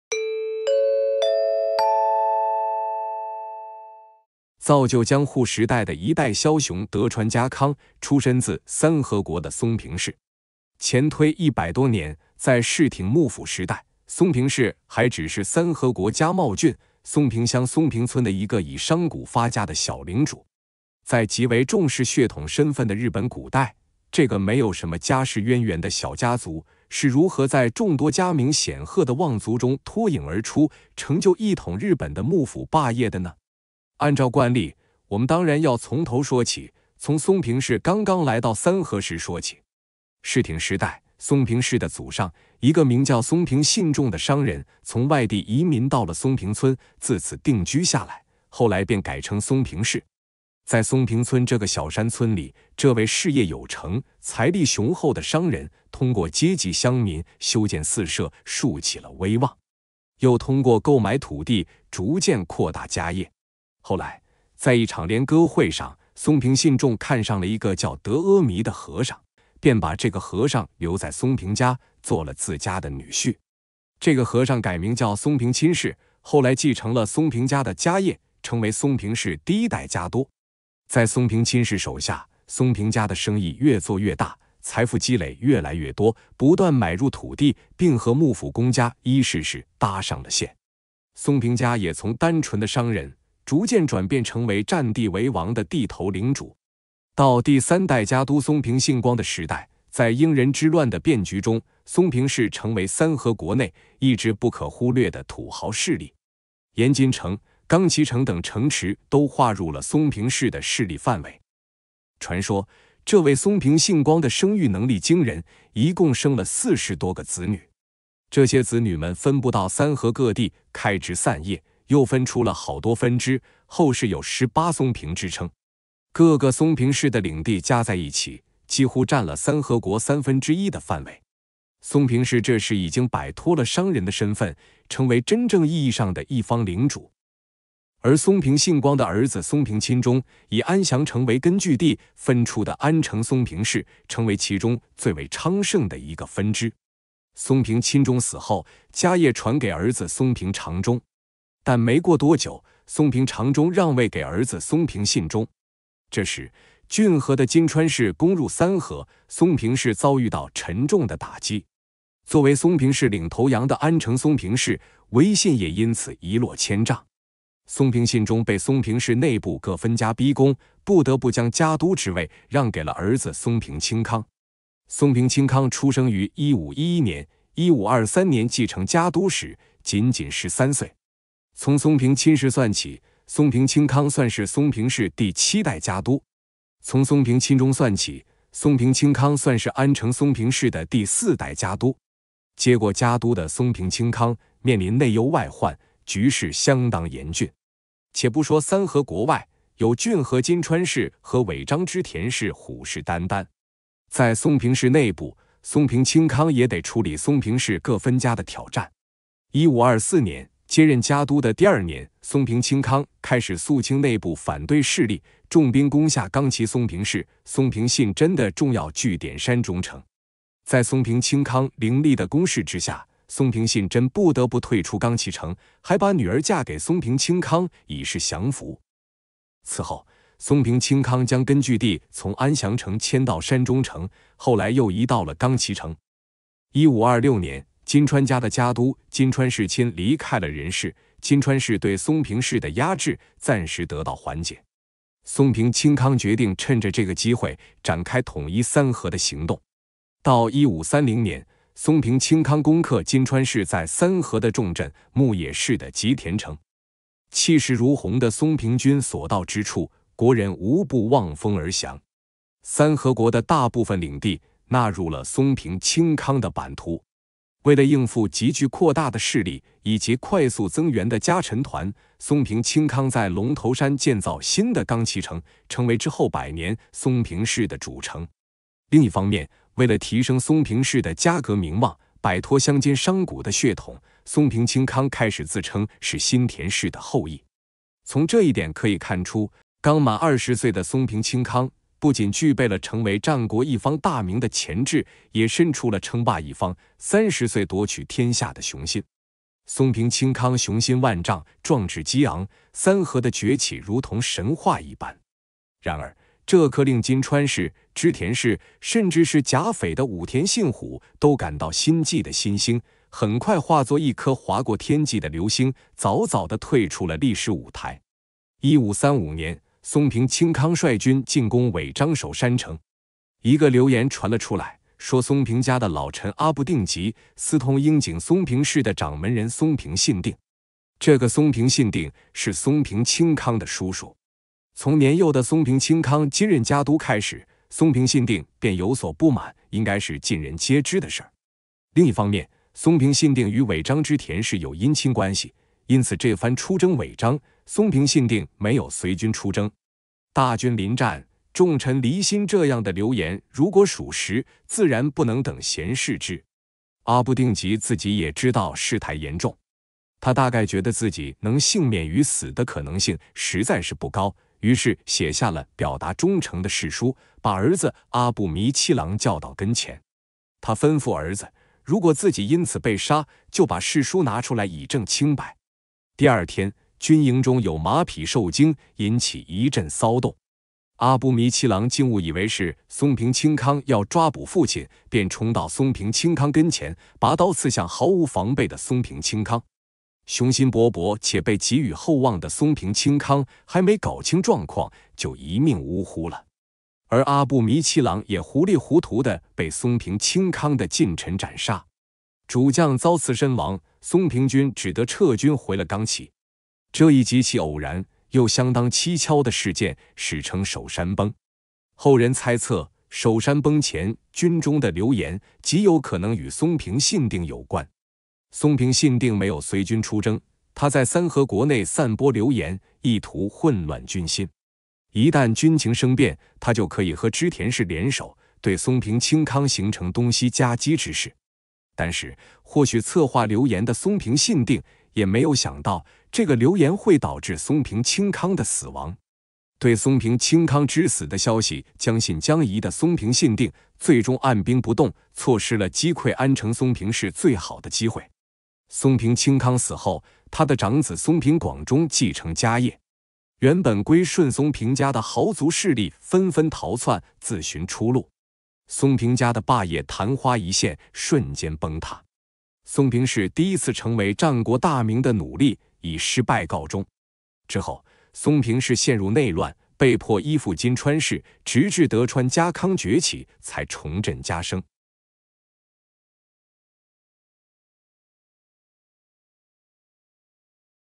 有三十歲奪取天下之志，戰斗還未開始，突然被終結的松平清康
造就江戶時代的一代梟雄德川家康，出身自三河國的松平氏。前推一百多年，在室町幕府時代，松平氏還只是三河國加茂郡，松平鄉松平村的一個以商賈發家的小領主。我們從頭說起，從松平氏剛剛來到三河時說起。藤家次